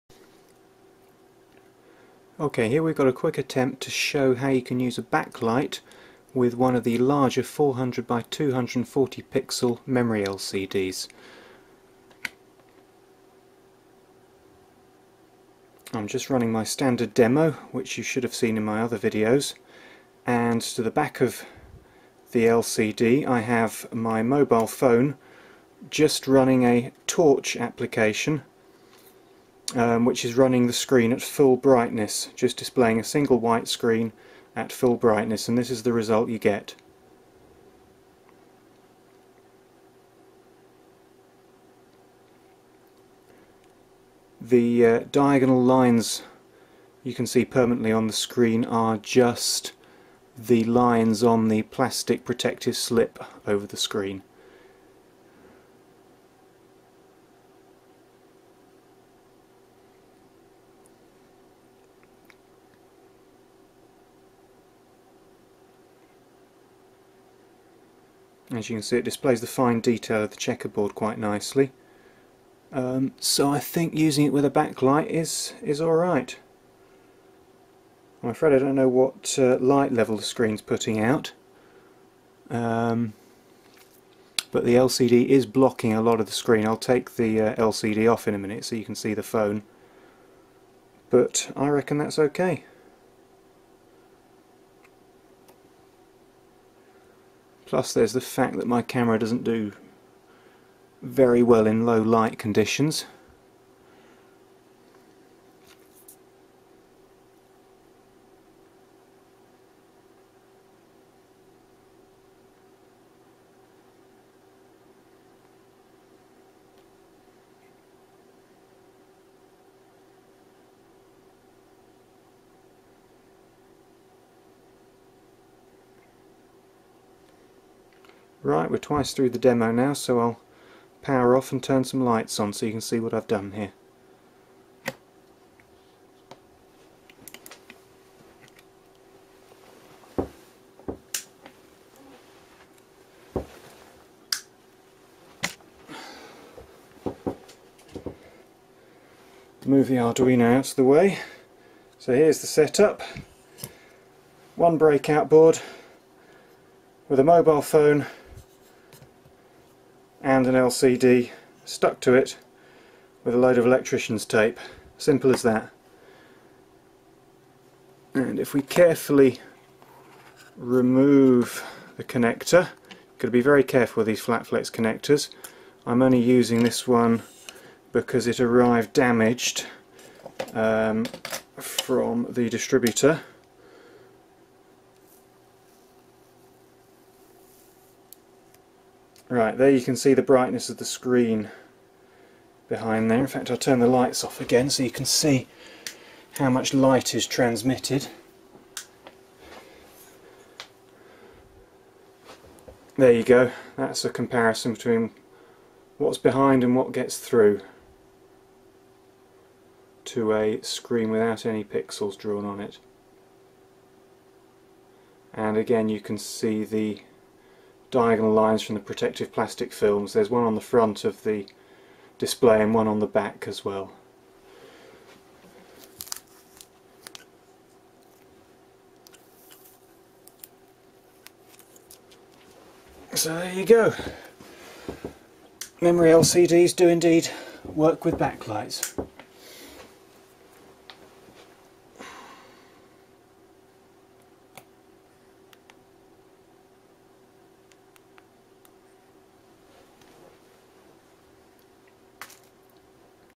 Memory LCD Backlight Test
A quick and dirty demonstration of a backlit Sharp Memory LCD.

The backlight is nothing more than a mobile phone running a "torch" app - where the screen is set to white and to maximum brightness. The LCD is fixed directly to the phone's screen using strips of electrical tape. The display used is the 400x240 pixel HR-TFT Memory LCD.

PLEASE NOTE: Use of a backlight is not possible by default with the breakout boards I sell as the LCD is normally glued to the PCB using double-sided sticky pads in the area where a backlight would need to be installed.